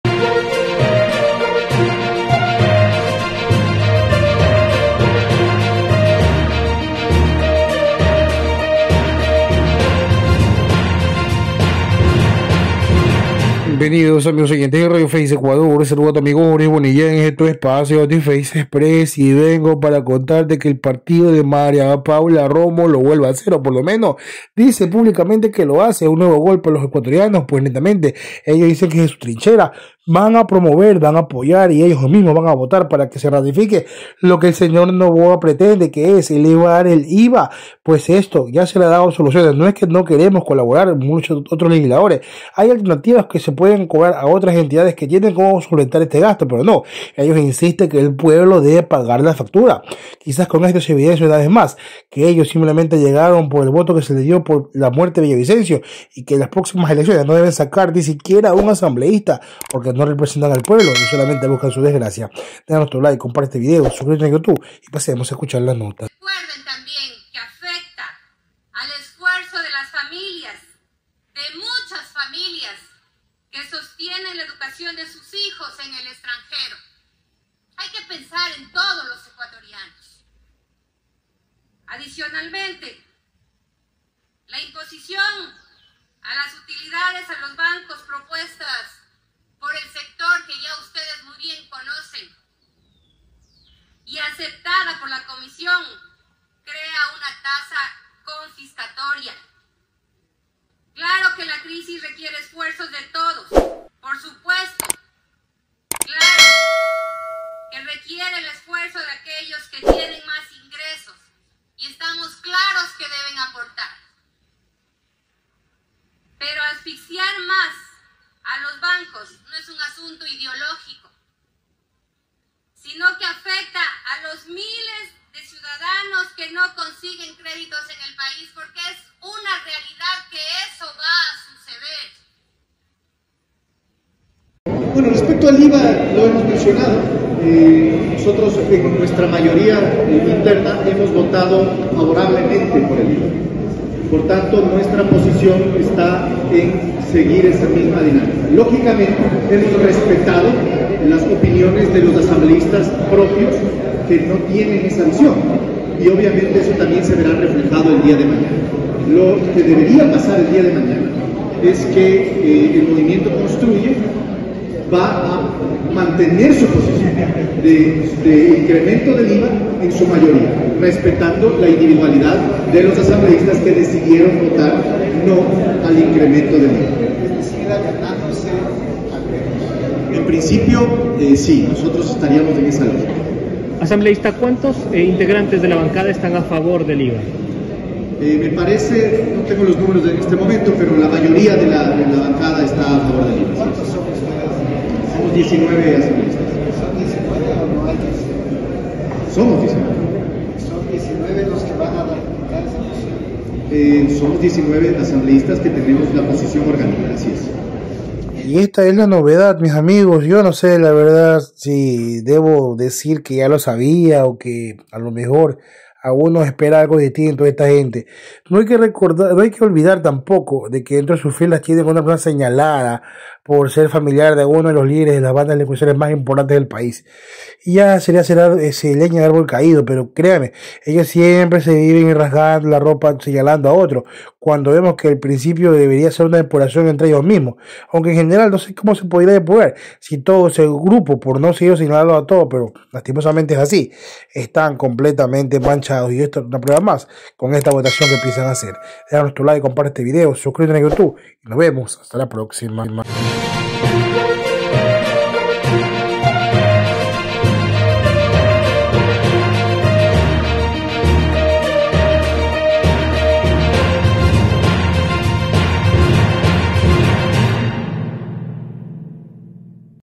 MARIA PAULA ROMO Y CONSTRUYE APOYARÁN NUEVO PAQU.ETAZO AL PUEBLO DE SUBIDA DEL IVA NOTICIAS RFE TV
MARIA PAULA ROMO SACA LAS GARRAS Y JUNTOS A SU BLOQUE DE CONSTRUYE APOYARÁN LA SUBIDA DEL IVA COMO PRETENDE NOBOA NOTICIAS RFE TV 👇